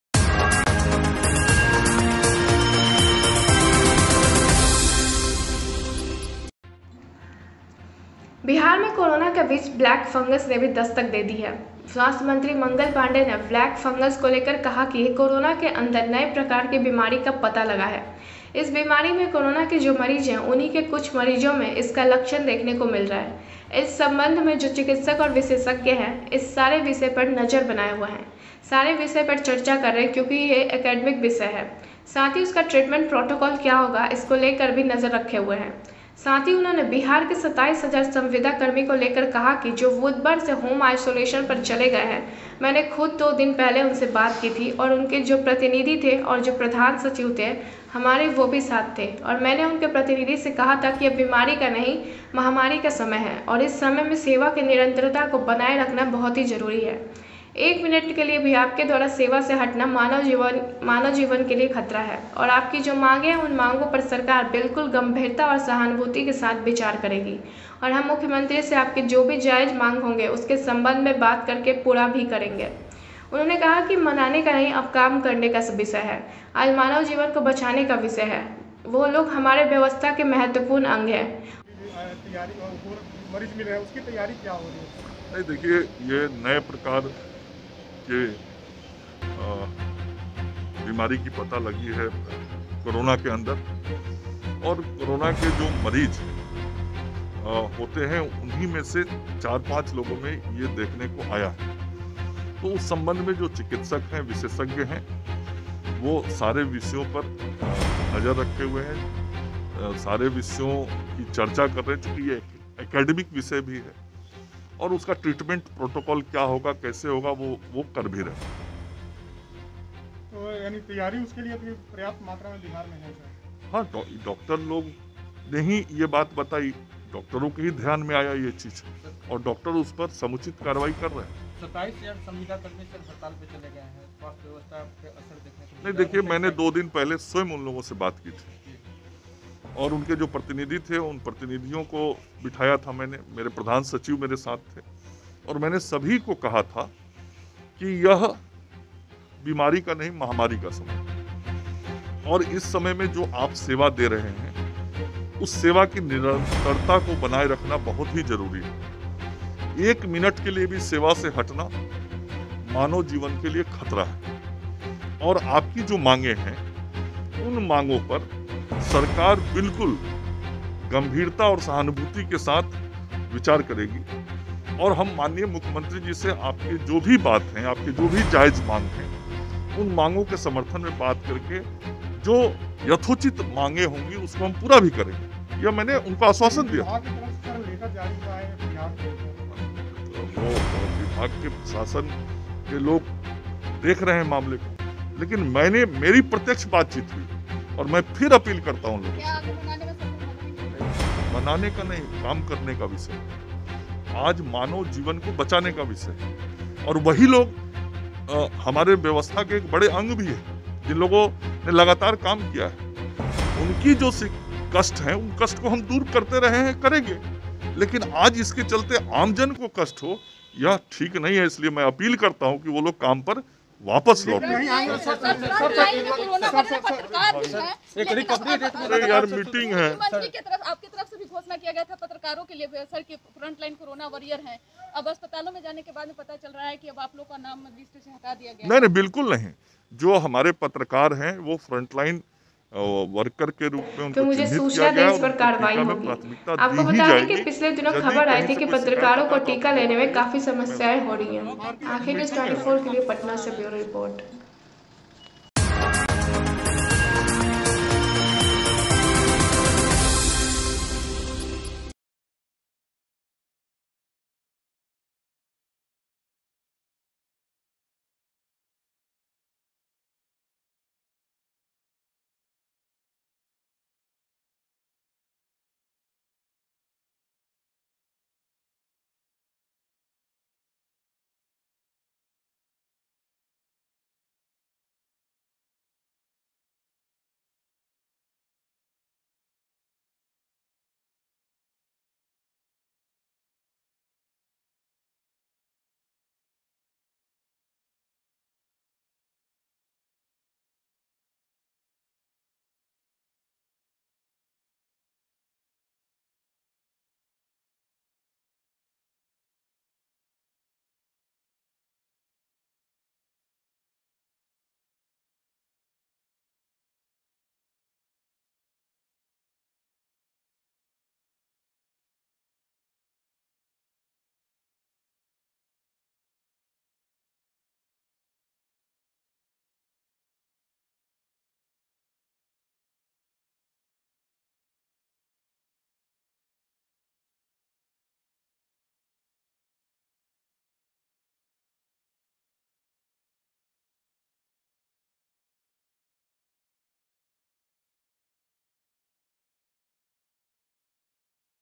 स्वास्थ्य मंत्री मंगल पांडेय- बिहार में कोरोना के बीच ब्लैक फंगस ने भी दस्तक दे दी है ।
बिहार में कोरोना के बीच ब्लैक फंगस ने भी दस्तक दे दी है। स्वास्थ्य मंत्री मंगल पांडेय ने ब्लैक फंगस को लेकर कहा कि ये नए प्रकार की बीमारी का पता लगा है कोरोना के अंदर । इस बीमारी में कोरोना के जो मरीज हैं उन्हीं के,कुछ मरीजों में इसका लक्षण  देखने को मिला है । इस संबंध में जो चिकित्सक और विशेषज्ञ हैं,इस सारे विषय पर नज़र बनाये हुए हैं । सारे विषय पर चर्चा कर रहें हैं क्योंकि ये अकैडमिक विषय है । साथ ही उसका ट्रीटमेंट प्रोटोकाल क्या होगा उसको लेकर भी नजर रखे हैं । साथ ही उन्होंने बिहार के 27 हज़ार संविदा कर्मी को लेकर भी कहा कि जो बुधवार से होम आइसोलेशन पर चले गए हैं ।  उन्होंने कहा कि मैंने खुद दो दिन पहले उनसे बात की थी और उनके जो प्रतिनिधि थे , वो मेरे साथ थे, जो प्रधान सचिव थे, हमारे वो भी साथ थे । और मैंने जो उनके प्रतिनिधि थे उनसे कहा था यह बीमारी का नहीं महामारी का समय है । और इस समय आप जो सेवा दे रहें हैं इस समय में सेवा के निरंतरता को बनाये रखना बहुत ज़रूरी है । एक मिनट के लिए भी आपके द्वारा सेवा से हटना मानव जीवन के लिए खतरा है । और आपकी जो मांगे हैं उन मांगों पर सरकार बिल्कुल गंभीरता और सहानुभूति के साथ विचार करेगी । और हम मुख्यमंत्री से आपके जो भी जायज माँग ह, उसके संबंध में बात कर के  मांगे पूरा भी करेंगे । उनको मैंने यह आश्वासन दिया था । उन्होंने कहा कि मनाने का नहीं अब काम करने का विषय है । आज मानव जीवन को बचाने का विषय है । वही लोग हमारे व्यवस्था के महत्वपूर्ण अंग है । उनलोगों ने लगातार काम करते रहें हैं । उनकी जो कष्ठ है उसको हम दूर करते हैं और करेंगे भी । आज इसके कारण आम जनों को कष्ठ हो यह ठीक नहीं है । मैं फिर से अपील करता हूँ कि काम पर वापस लौट जाए । साथ ही उन्होंने कहा कि पत्रकारों के वैक्सीनेशन के सवाल पर कहा कि पत्रकार फ्रंटलाइन वर्कर है अगर उन्हें टिका लेने में कोई समस्या आती है तो वो मुझे सूचना दें इसपर कार्रवाई होगी । आपको बता दें कि पिछले दिनों खबर आई थी कि पत्रकारों को टिका लेने में समस्या आ रही है ।
Welcome to Aankhen News 24 Channel.